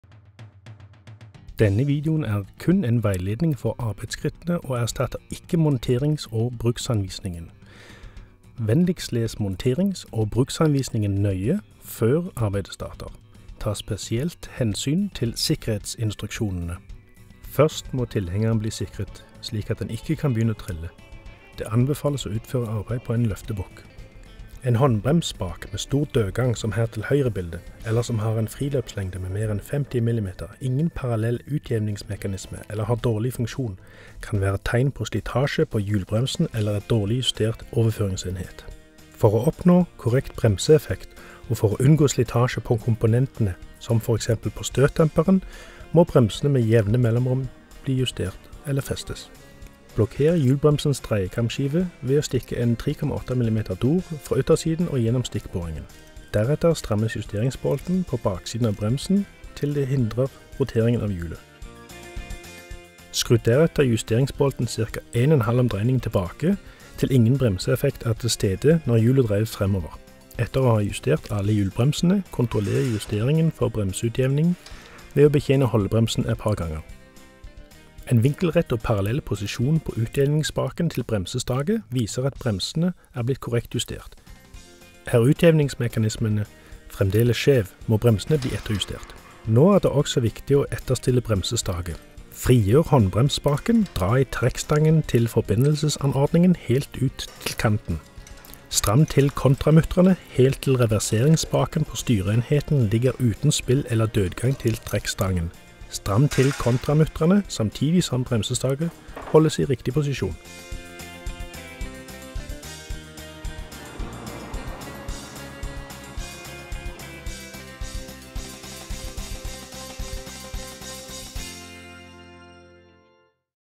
Innstille BPW-bremseanlegg
For å oppnå korrekt bremseeffekt og for å unngå slitasje på komponentene, som for eksempel på støtdemperen, må bremsene med jevne mellomrom bli justert/festes.